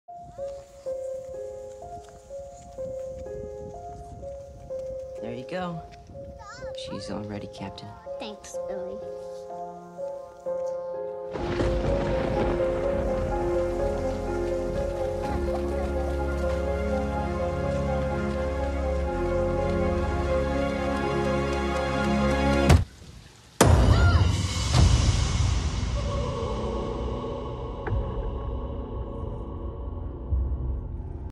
Hazelle as Georgie from It 2017
Hazelle was wearing a yellow raincoat today and the resemblance was uncanny.  It was especially strange that our neighbors had balloons outside on their porch.  We had a little fun with it.